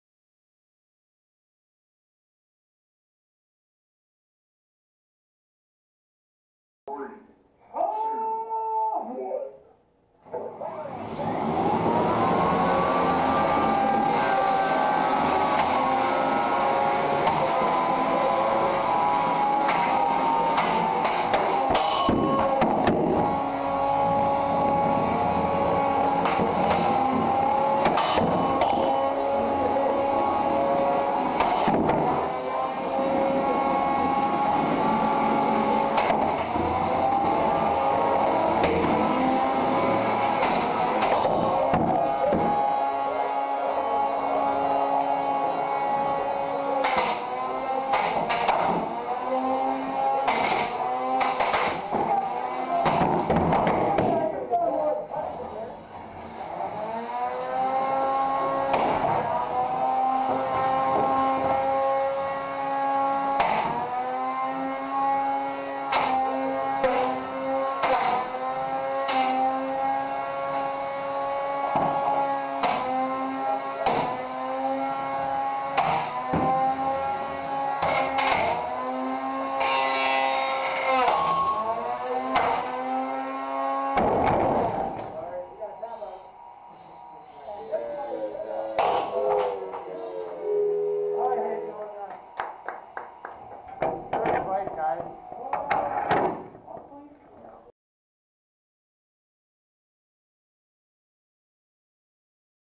BotBrawl 1lb: Beez Nuts vs Left Foot Right Shoe
Always make sure you have the right shoe on other wise you might be Left Foot Right Shoe. What do you get when you mix a bee and a robot? Beez Nuts!

Event: BotBrawl Backyard Battles 2021

Come and join the League.